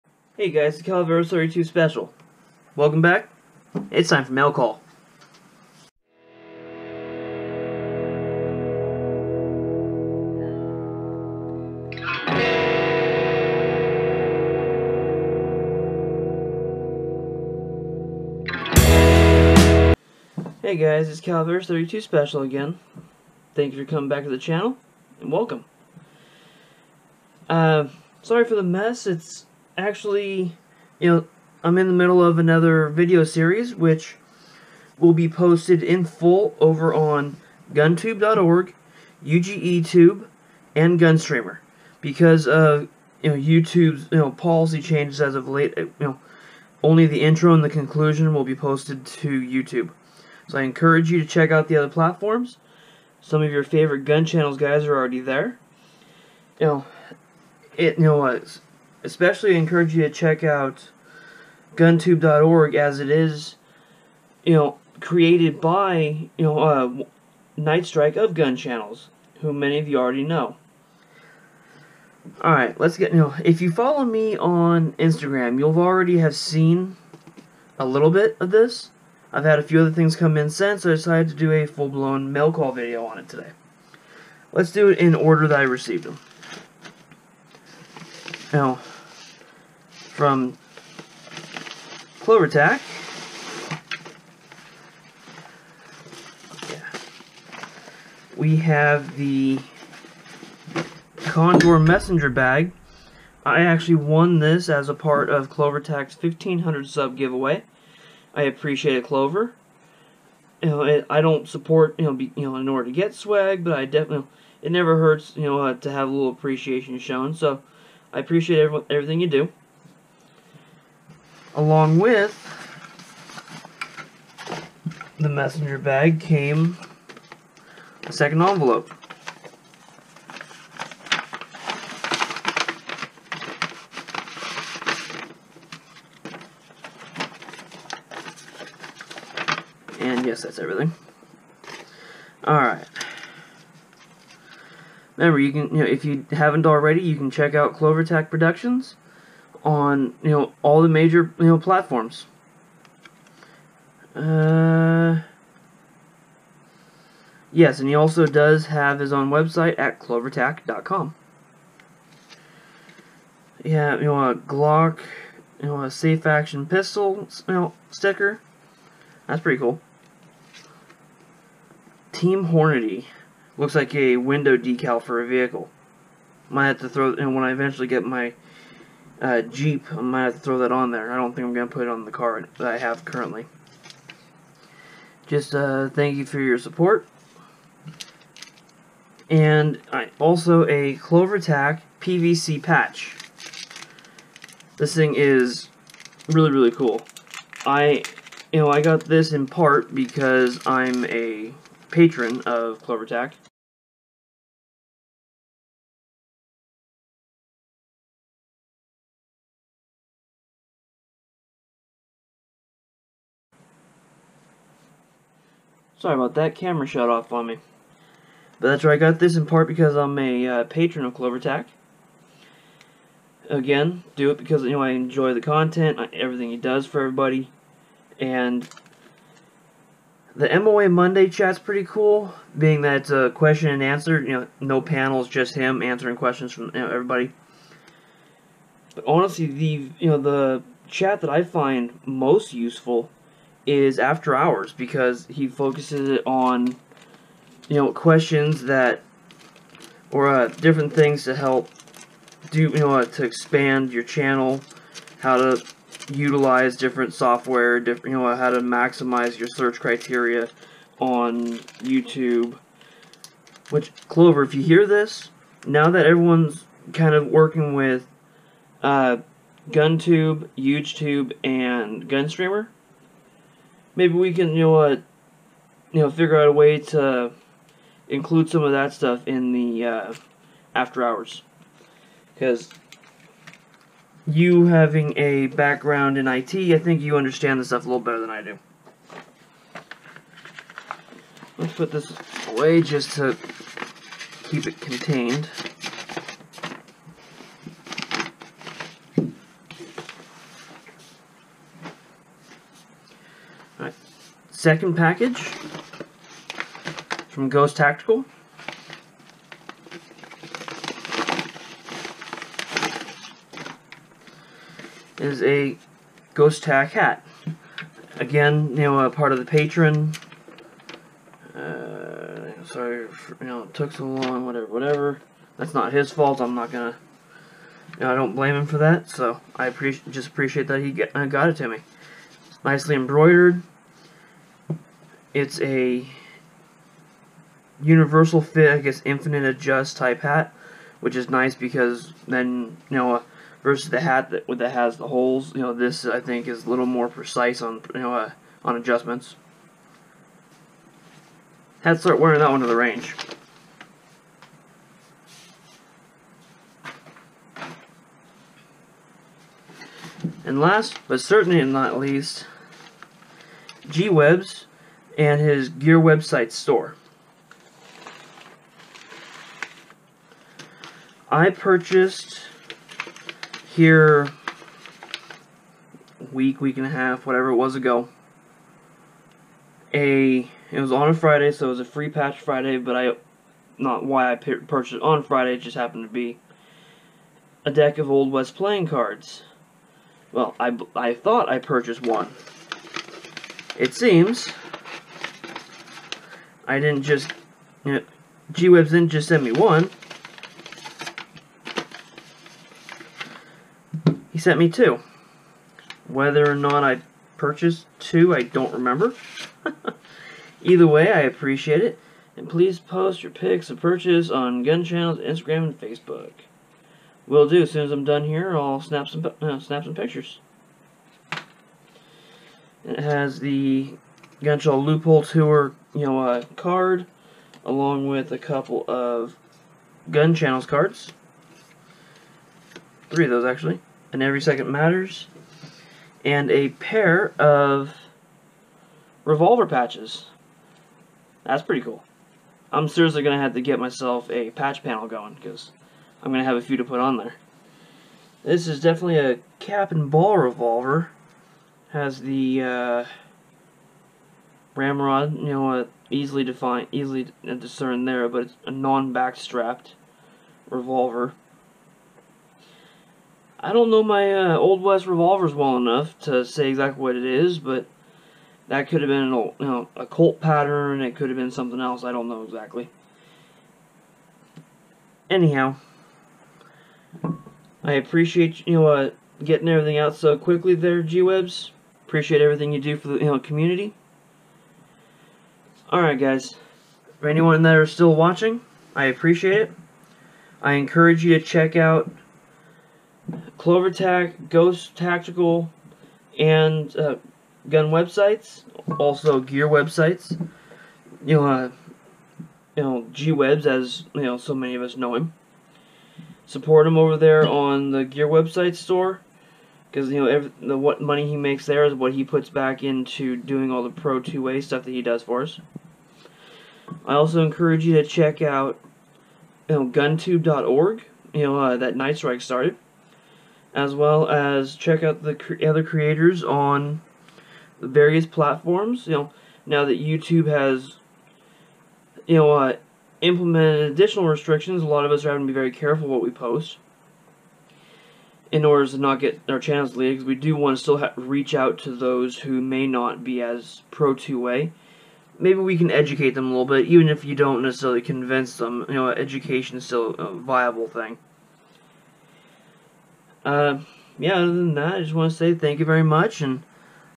Mail Call - Ghost Tactical, Clovertac, GearWebsites
A quick Mail Call video with some really cool items from Clovertac, Ghost Tactical and GearWebsites.